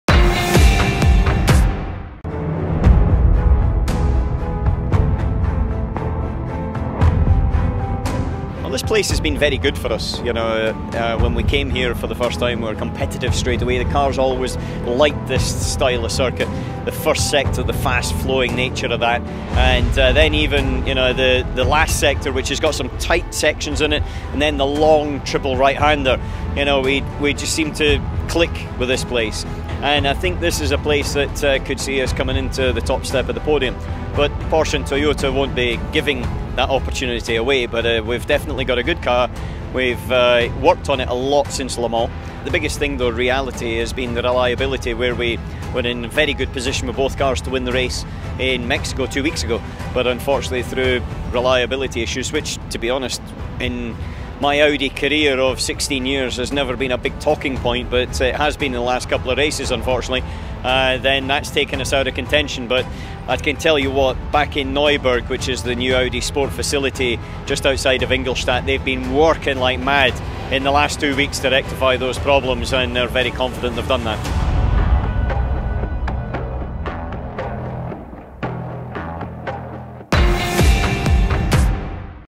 McNish on Austin
Allan McNish - multiple Le Mans winner with Audi - is at Austin supporting the team. We asked about their chances - given a rough season start.